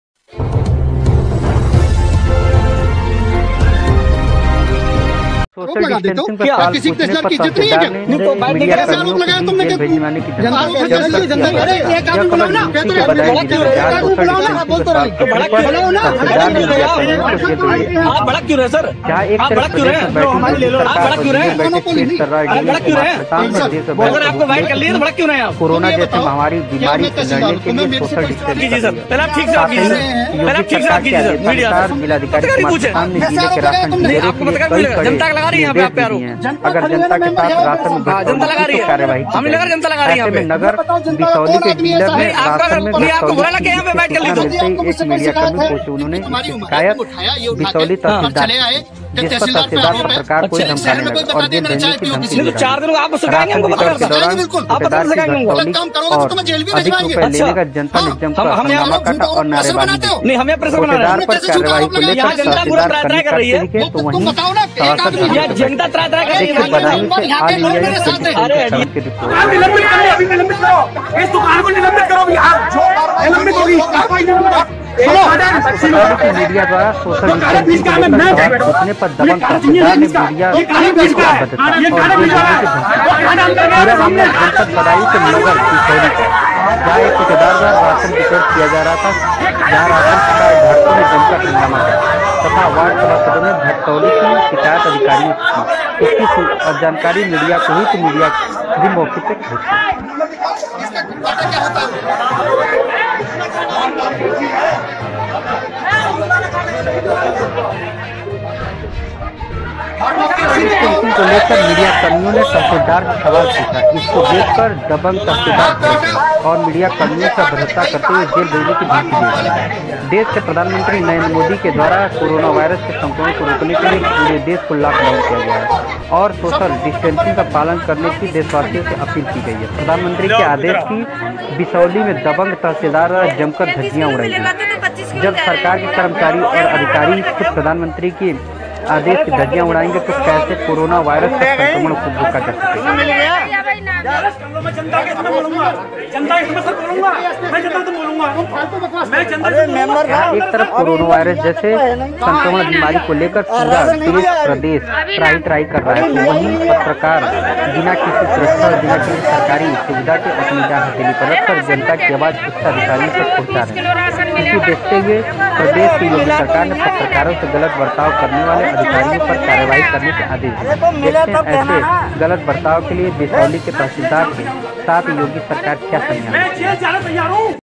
सोशल डिस्डेसिंग पर सवाल पूछने पर तहसीलदार ने मीडिया कर्मियो को जेल भेजने की दी धमकी जमकर की अभद्रता
सोशल डिस्डेसिंग | All India News

सोशल डिस्डेसिंग पर सवाल पूछने पर तहसीलदार ने मीडिया कर्मियो को दी जेल भिजवाने  की धमकी जम कर की अभद्रता

एंकर= बिसौली(बदायूँ)एक तरफ प्रदेश में बैठी योगी सरकार और जिले में बैठे तेजतर्रार डीएम कुमार प्रशांत ने देश मे भयंकर रूप ले चुकी कोरोना जैसी महामारी विमारी फैल चुकी है ।दूसरी तरफ योगी सरकार के आदेशानुसार जिलाधिकारी कुमार प्रसांत ने जिले के राशन डीलरो के लिए कड़े निर्देश दिए की अगर जनता के राशन में घटतौली की तो कार्यवाही की जाएगी ।ऐसे में नगर बिसौली के डीलर ने राशन में घटतौली की बहा एक मीडिया कर्मी पहुँचे उन्होने उसकी शिकायत बिसौली तहसील दार से की जिस पर तहसीलदार पत्रकार को ही धमकाने लगे और जेल भेजने की धमकी दे डाली । राशन वितरण के दौरान कोटेदार की घटतौली और रूपए अधिक लेने का जनता ने जमकर हंगामा काटा और नारेबाजी भी की। कोटेदार पर कार्रवाई को लेकर तहसीलदार कन्नी काटते दिखे तो सभाषद धरने पर बैठ गये।

वी/ओ= मीडिया द्वारा सोशल डिस्टेन्सिंग को लेकर सवाल पूंछने पर दबंग तहसीलदार ने मीडिया से की अभद्रता और दी जेल भेजने की धमकी। पूरा मामला जनपद बदायूं के नगर बिसौली का जहां एक कोटेदार द्वारा राशन वितरण किया जा रहा था जहां राशन कार्ड धारकों ने जमकर हंगामा काटा तथा वार्ड सभाषदो ने घटतौली की शिकायत अधिकारियों से की। इसकी जानकारी मीडिया को हुई तो मीडिया भी मौके पर पहुंच गई।
सोशल डिस्टेंस को लेकर मीडिया कर्मियों ने तहसीलदार से सवाल पूछा इसको लेकर दबंग तहसीलदार भड़क गया और मीडिया कर्मियों से अभद्रता करते हुए जेल भेजने की धमकी भी दे डाली।  देश के प्रधानमंत्री नरेंद्र मोदी द्वारा कोरोनावायरस के संक्रमण को रोकने के लिए पूरे देश को लॉकडाउन किया गया है और सोशल डिस्टेंसिंग का पालन करने की देशवासियों से अपील भी की। प्रधानमंत्री के आदेश की बिसौली के दबंग तहसीलदार जमकर धज्जियां उडा रहे। जब सरकार के कर्मचारी और अधिकारी ही खुद प्रधानमंत्री के आदेश की धज्जियां उड़ायेगे तो कैसे कोरोनावायरस का संक्रमण रोका जा सकेगा।बहि एक तरफ कोरोना बायरस जैसे संक्रमण बीमारी को लेकर पूरा देश प्रदेश त्राहि त्राहि कर  रहा है ।तो बहि पत्रकारो बिना किसी सुरक्षा और बिना किसी सरकारी सुविधा के अपनी जान हथेली पर रखकर जनता की आबाज उच्चाधिकारियों तक पहुँचा रहे है ।इसे देखते हुए प्रदेश की योगी सरकार ने पत्रकारों से गलत बर्तव्य करने बाले अधिकारियों पर कार्यवाही करने के आदेश दिए है ।देखते है ऐसे गलत बर्तव्य के लिए बिसौली के तहसीलदार के साथ योगी सरकार क्या संज्ञान लेती है।